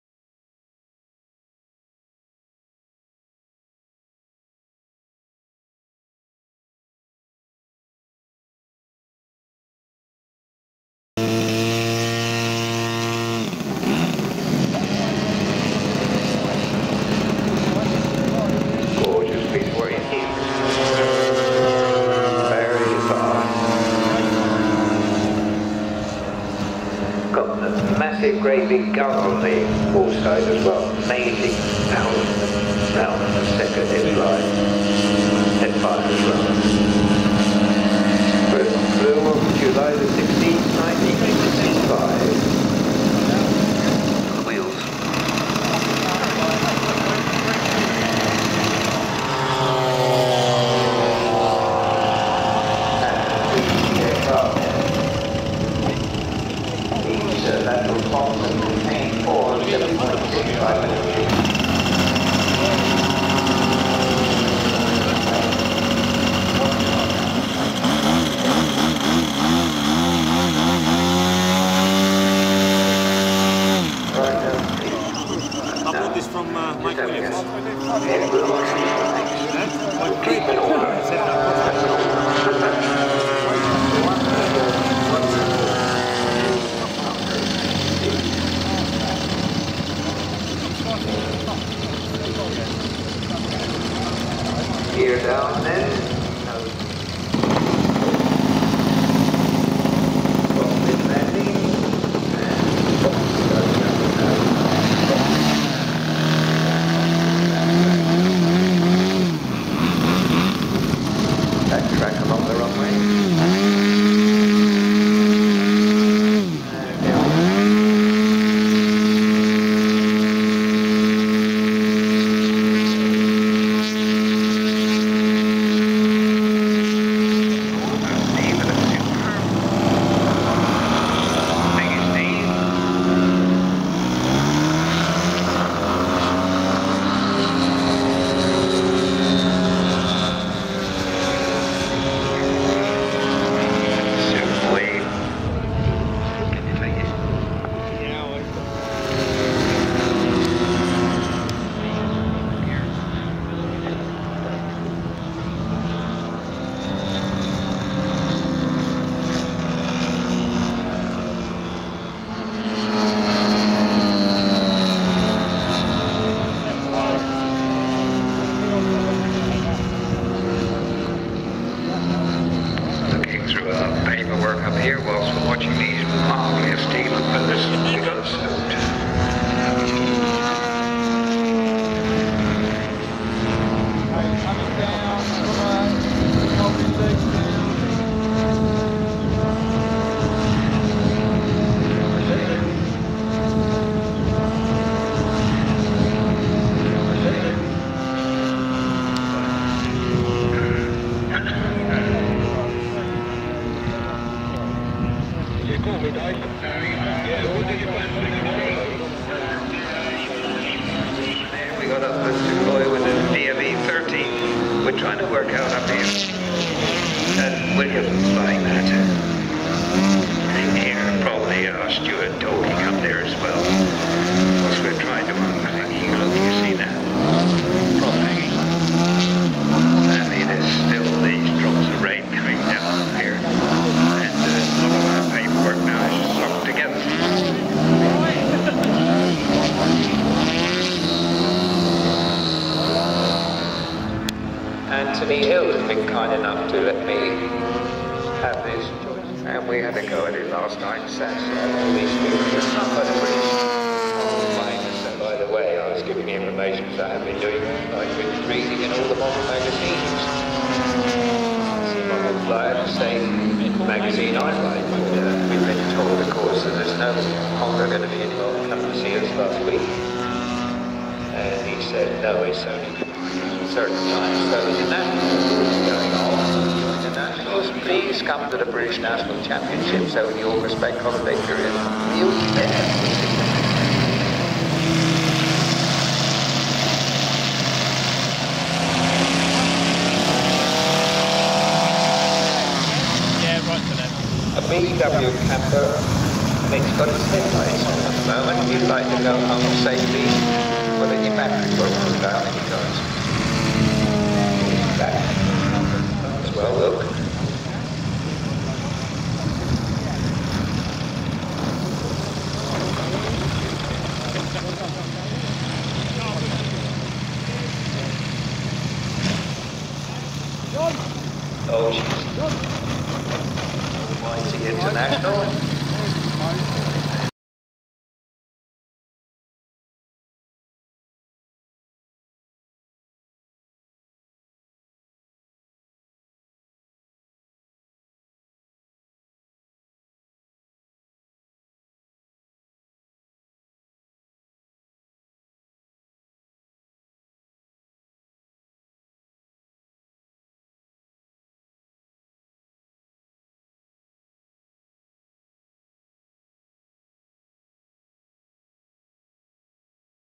RC 3D SLOT / DISPLAY AT WINGS & WHEELS MODEL AIRCRAFT SHOW - 2014
FILMED OVER THE WEEKEND OF JUNE 28th & 29th - 2014 AT THE WINGS & WHEELS MODEL SPECTACULAR AT NORTHWEALD AIRFIELD - EPPING - ESSEX - CM16 6AR ,
TURN OFF AT JUNCTION 7 ON THE M11,